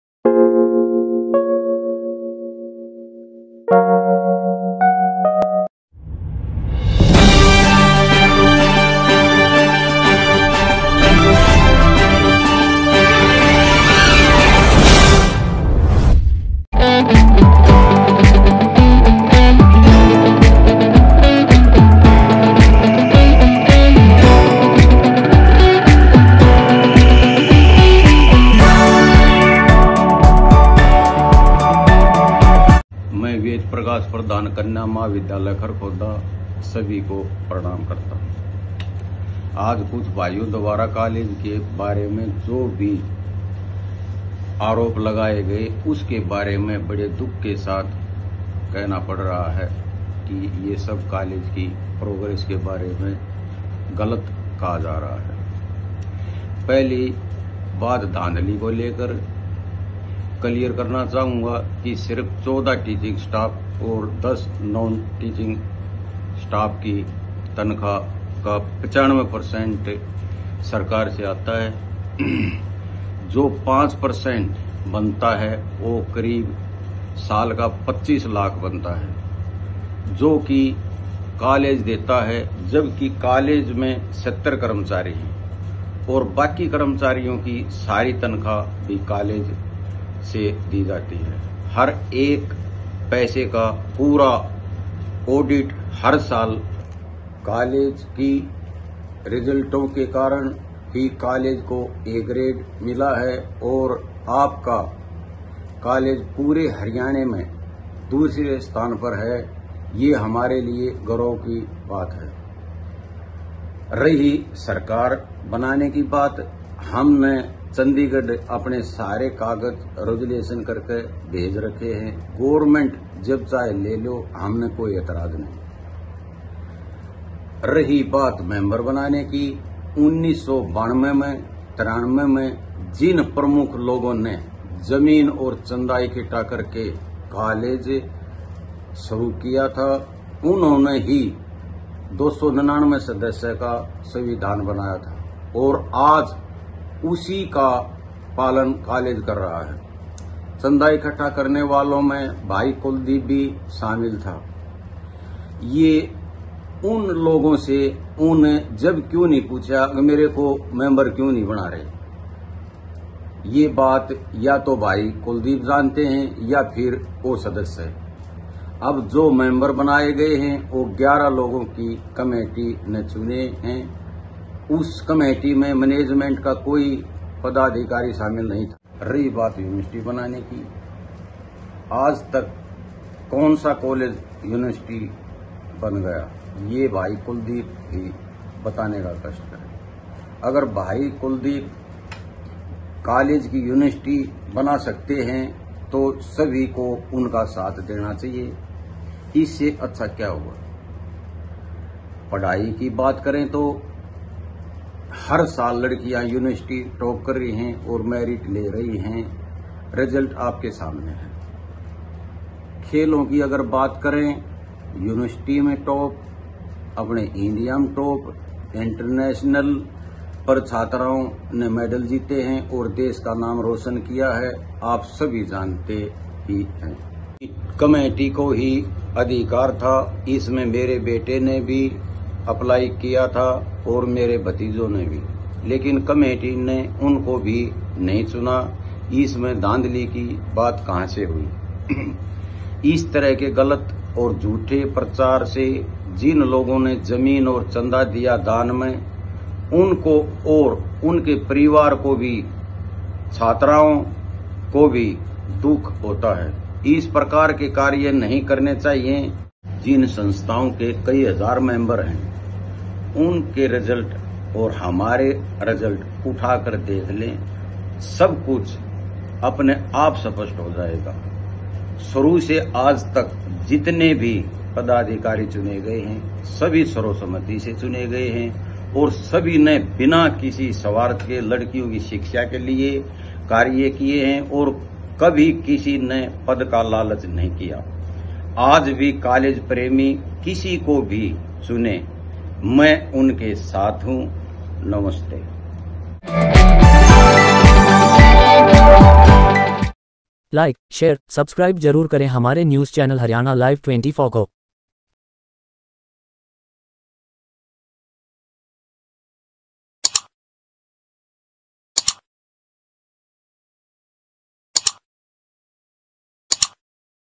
कन्या कॉलेज समिति पर लगे आरोपों पर बोले - प्रधान वेद प्रकाश।
खरखौदा कन्या कॉलेज समिति पर लगे आरोपों पर दिया जवाब - प्रधान वेद प्रकाश।